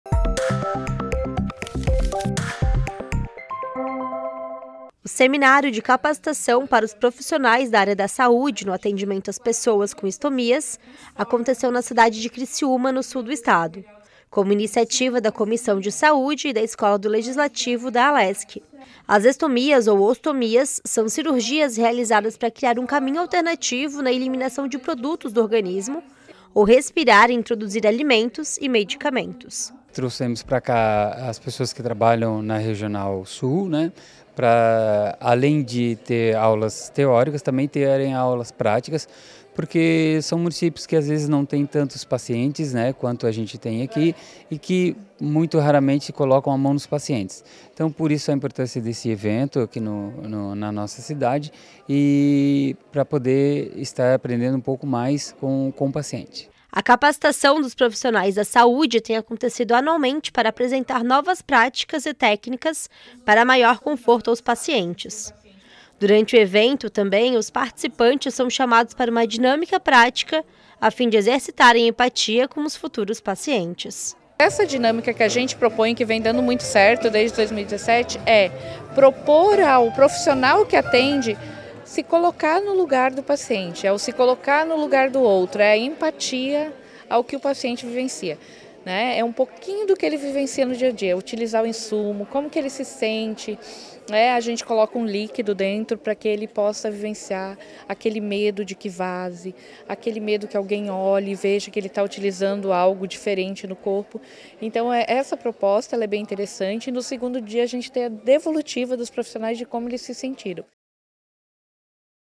É Notícia - Seminário capacita profissionais da saúde para atender pessoas com estomias - 28/06/23
A Assembleia Legislativa de Santa Catarina realizou, nesta terça-feira e quarta-feira, 27 e 28 de junho, o Seminário de Capacitação na Atenção à Saúde das Pessoas com Estomias, na cidade de Criciúma. O Seminário foi uma iniciativa da Comissão de Saúde, Escola do Legislativo e Secretaria de Estado da Saúde, com o objetivo de apresentar novas práticas e técnicas para o tratamento.